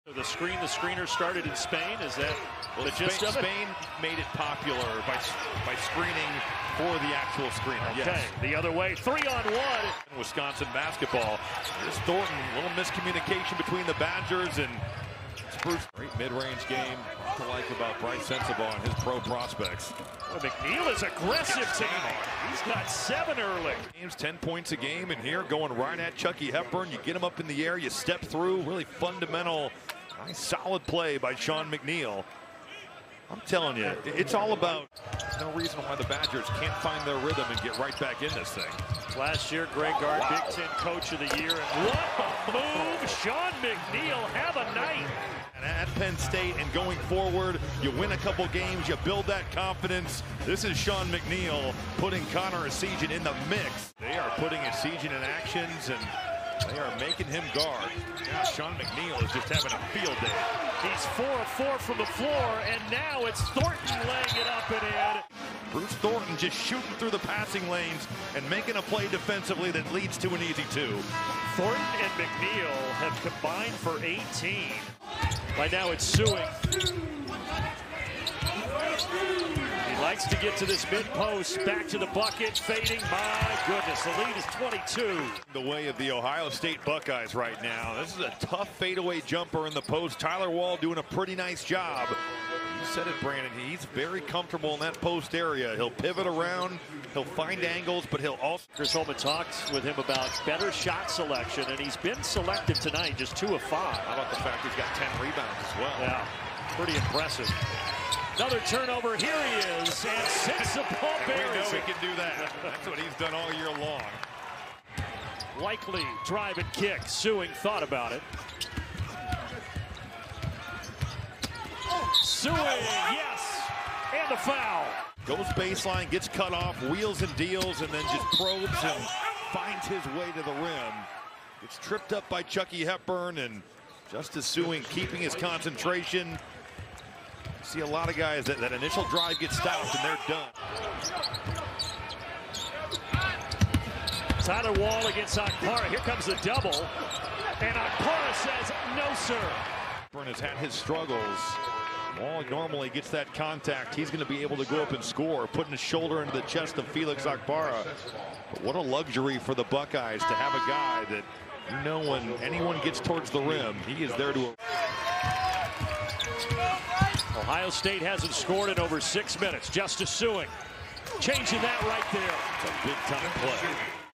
Ohio State Highlights against the Wisconsin Big Ten Tournament
Buckeyes roll out to big lead, hold on to advance to Day 2 in Chicago